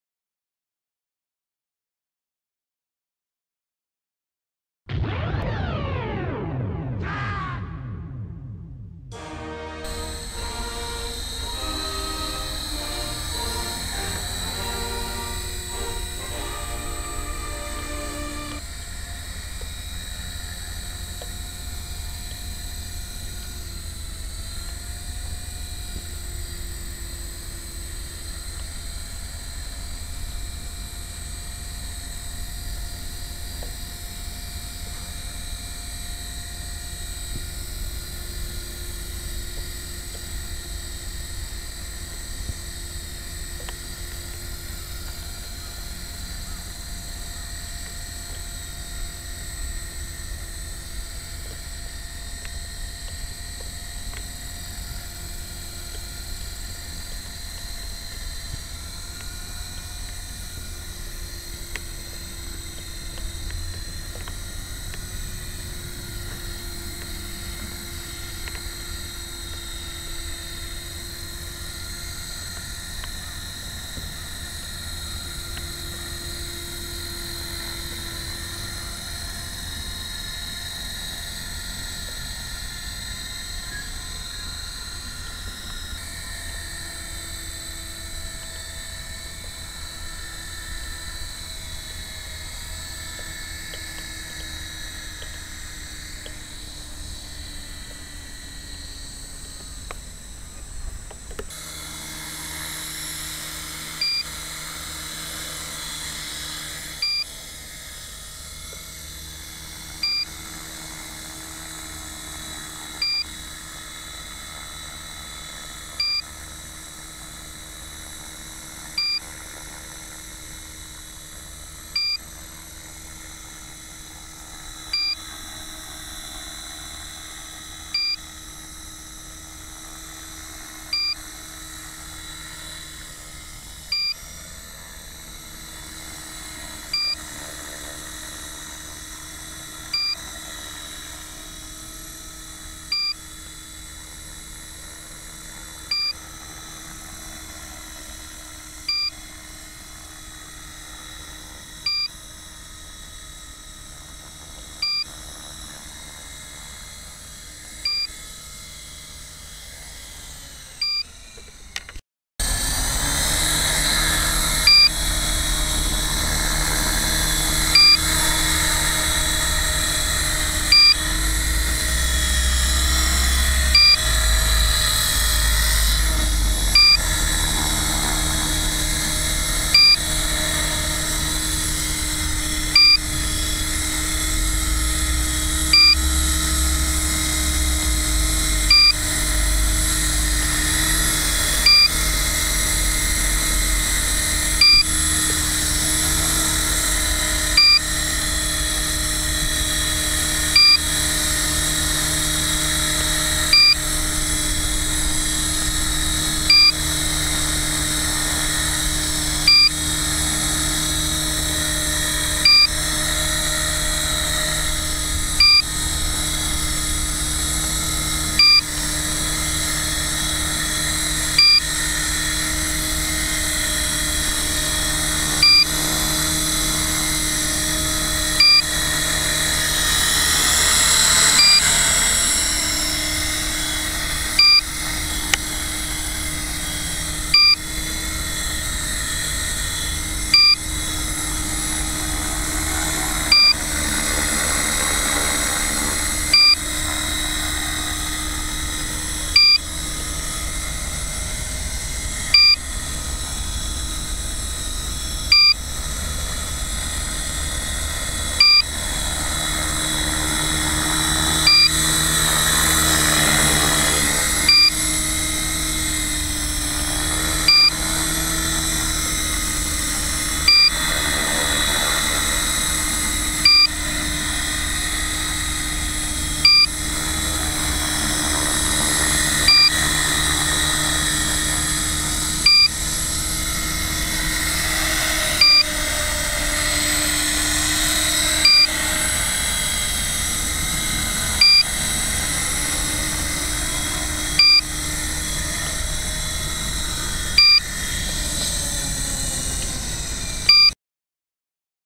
ヘリコプター練習2013/07/31
誰も居なくなった港の公園で。７月の最終練習、仕上げ。残骸から組んだらT-REX450PROになった。フライバーヘッドへ乗せ換えての初飛行。KDSイノーバは調子はいいのだが、ちょっと音が変わった。ギアまわりかな？